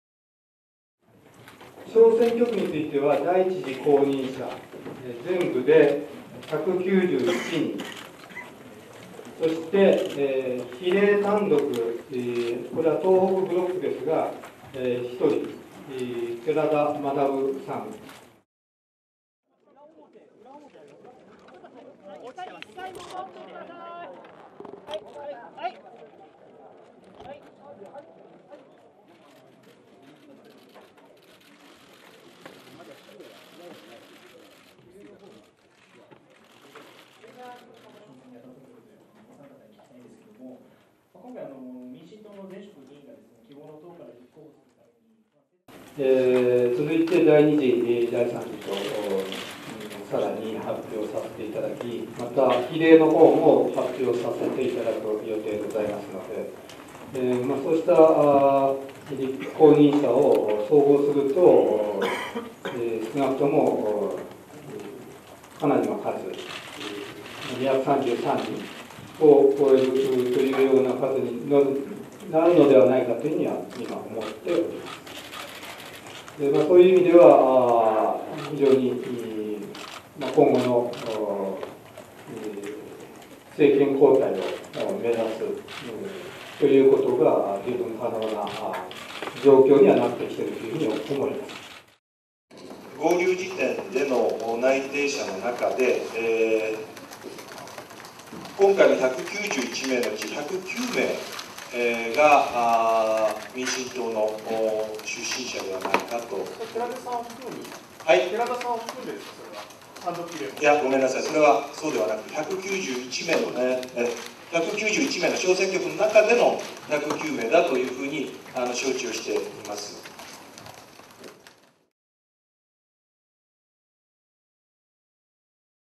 希望が１次公認１９２人 民進党出身者は１１０人
希望の党（代表・小池百合子東京都知事）は３日、衆院選（１０日公示―２２日投開票）の第１次公認候補として計１９２人（小選挙区１９１人、比例代表１人）を擁立すると発表した。合流に伴う民進党出身者は計１１０人。若狭勝前衆院議員は、公認された候補は全員、憲法改正に賛成する立場だとの認識を示した。